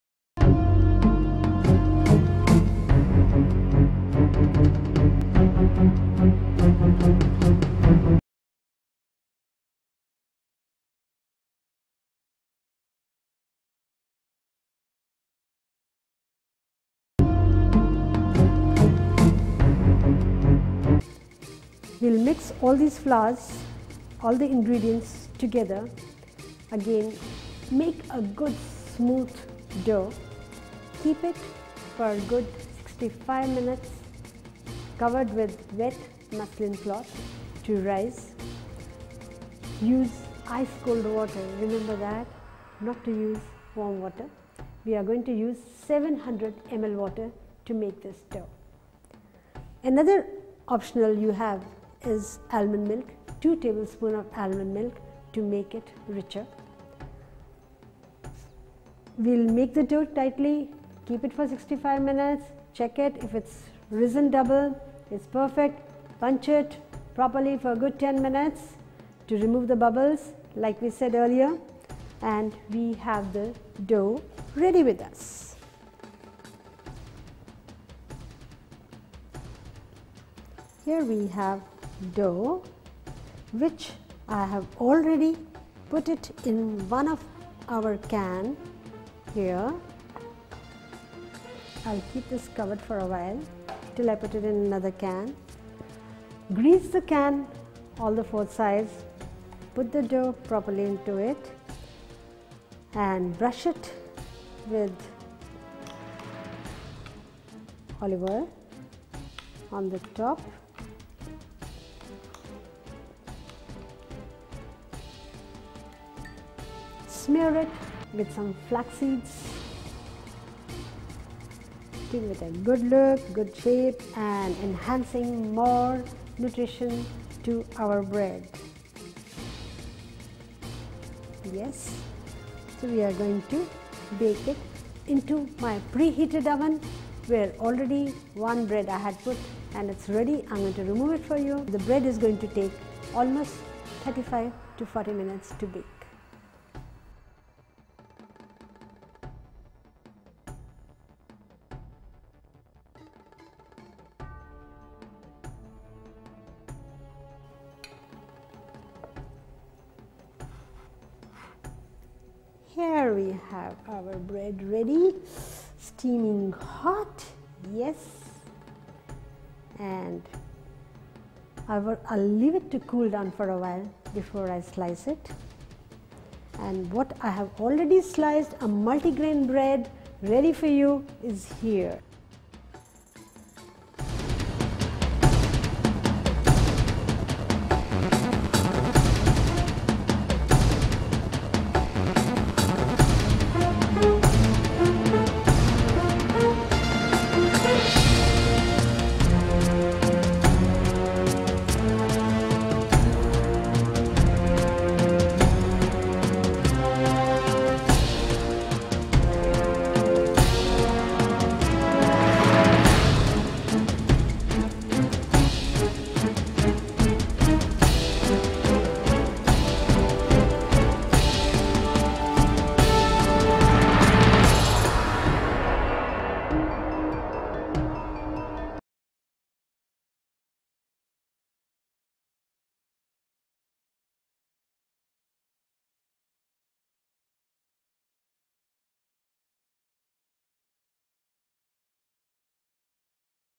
HOW TO MAKE MULTIGRAIN BREAD II HOMEMADE MULTIGRAIN BREAD EASY RECIPE
Homemade multigrain bread recipe.
Make your own Multigrain bread loaf at home. No need to buy bread from the market. Try this easy recipe now!